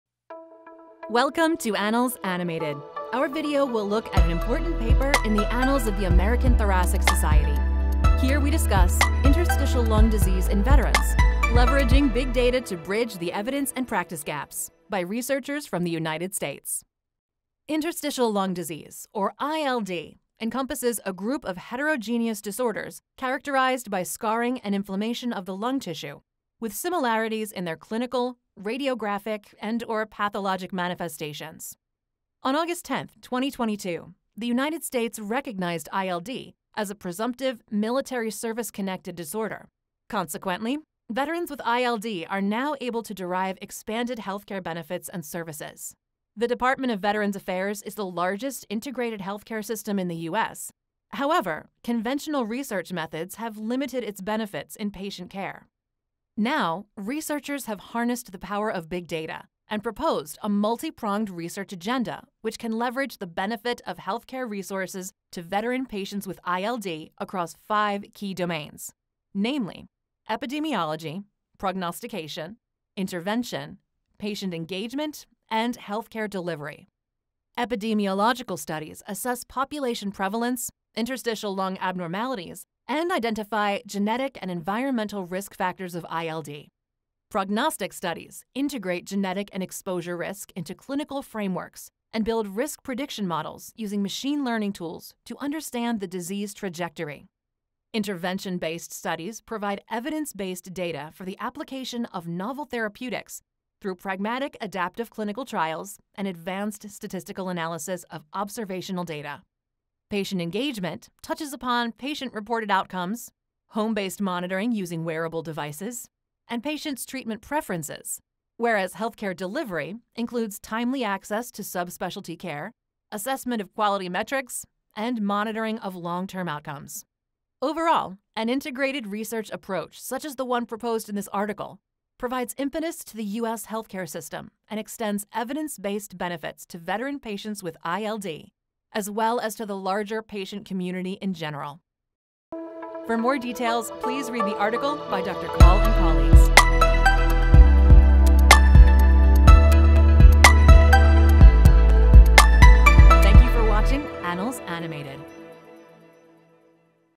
Interstitial Lung Disease in Veterans: Leveraging Big Data to Bridge the Evidence and Practice Gaps
U.S. government reform provides impetus to healthcare and extends benefits to veteran patients with interstitial lung disease (ILD). An article published in Annals of the American Thoracic Society proposes a multi-pronged research approach based on epidemiology, prognostication, intervention, patient engagement, and healthcare delivery that can enhance healthcare benefits to veteran patients with ILD.